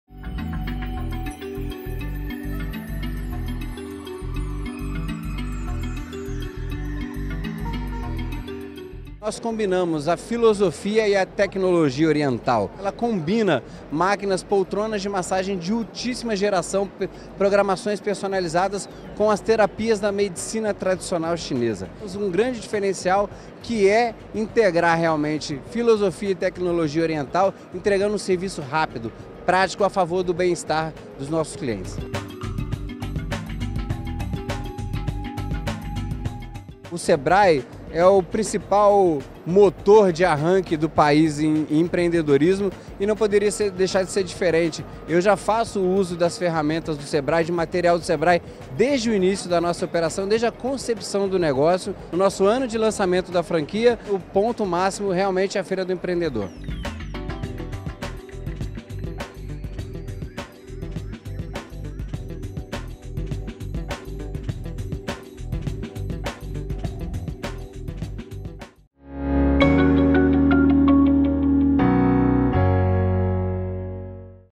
Spa Express é uma das opções de negócios disponíveis na Feira do Empreendedor 2019
A Feira do Empreendedor traz diversas opções de negócios para quem quer empreender.